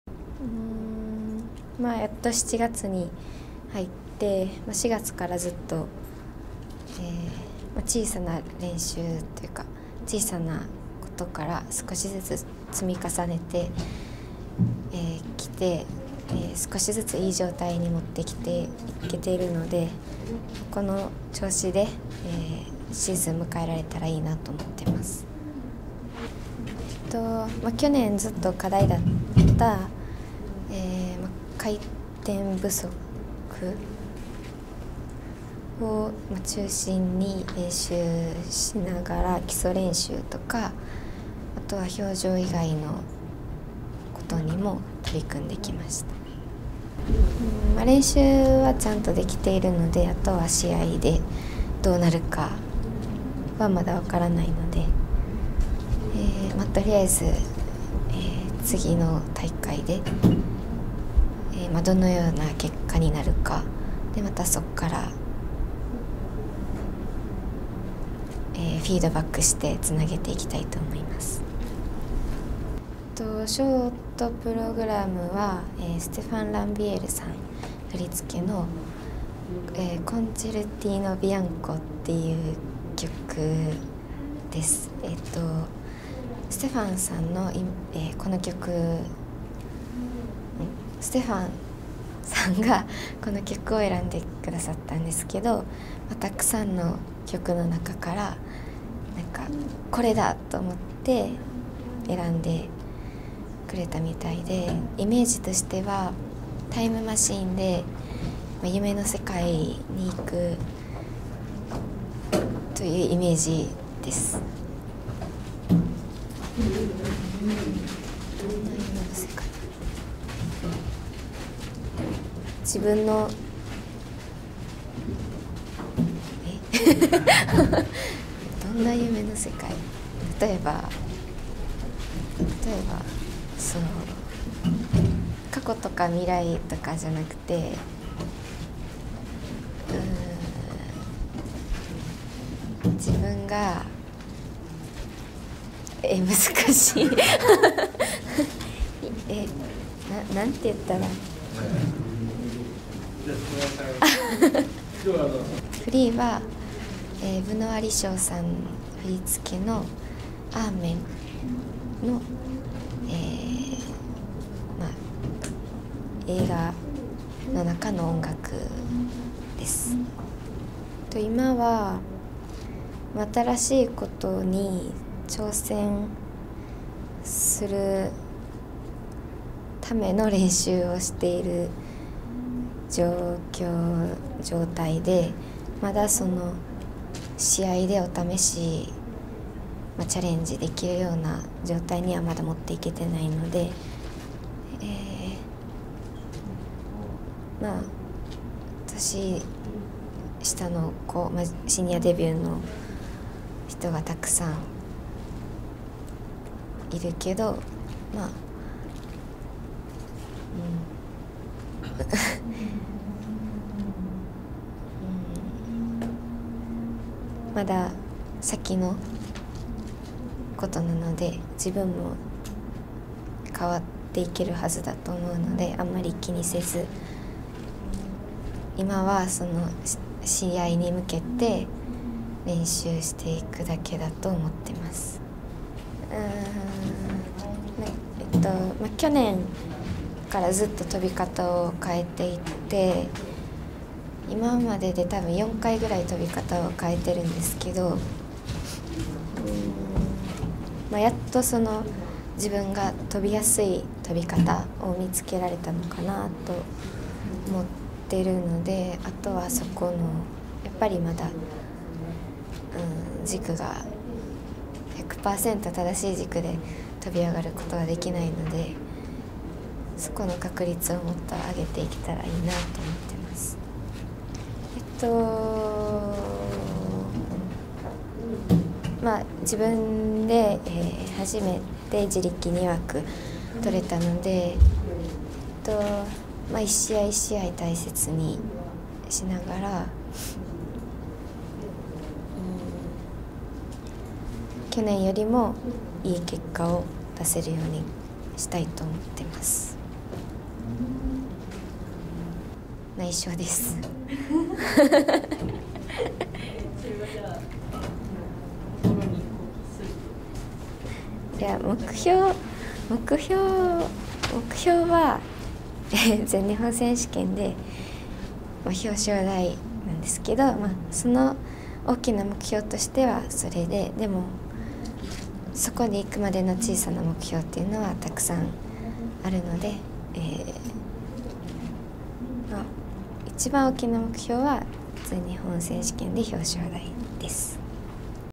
【フジテレビ公式】白岩優菜 シニア合宿
今年7月に取材させていただいた白岩優菜選手のシニア合宿でのインタビュー動画です。
2019-20シーズンはもう始まってる！

フジテレビ スケート総合サイト「フジスケ」